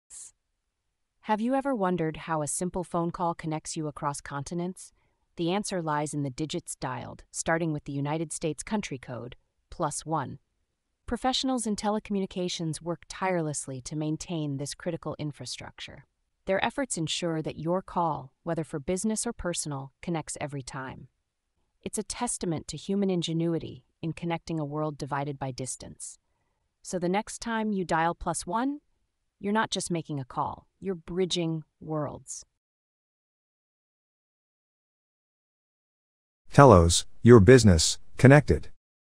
Cracking the Code: United States Phone Country Code Explained.
Calling someone in the United States and stuck on the phone country code? Don't sweat it! Teloz breaks it down for you in this informative video. Learn the US phone country code, understand how to dial internationally, and connect with anyone in the States with ease. Save money, avoid confusion, and become a dialing pro!